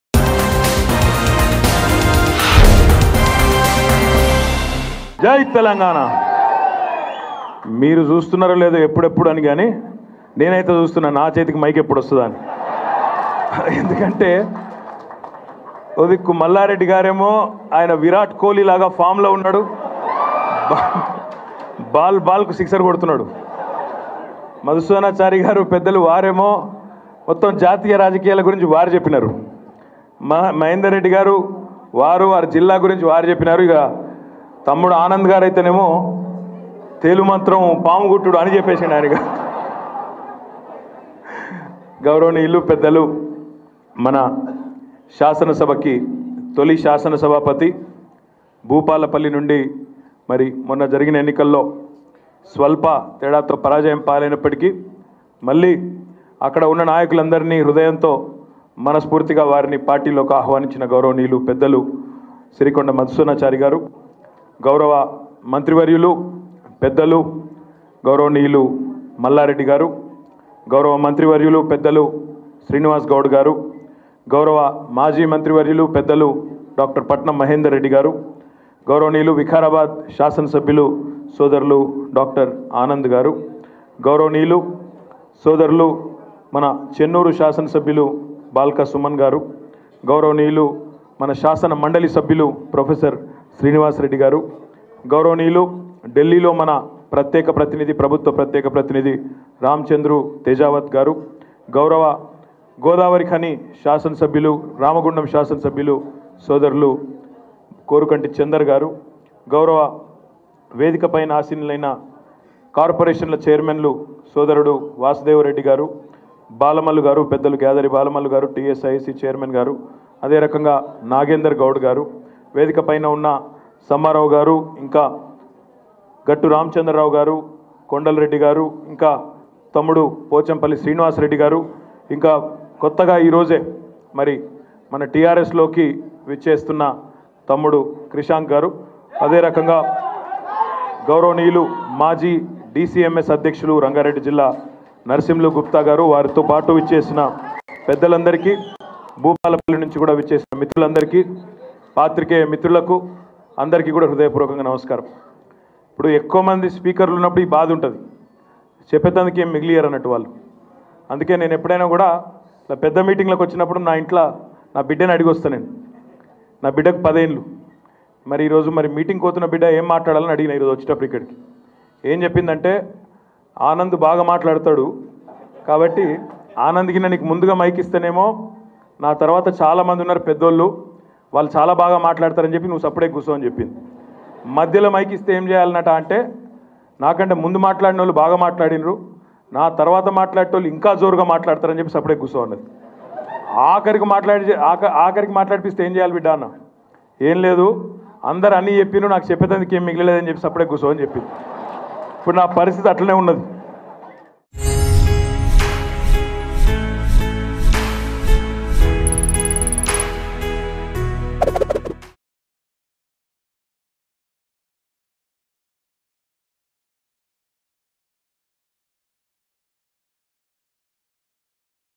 KTR Shares Funny Moments With His Daughter | TRS Meeting In Telangana Bhavan | Mango News
KTR Shares Funny Moments With His Daughter. He spoke about the incident happened with his daughter before coming to the event where she suggested him to keep quiet in the meeting.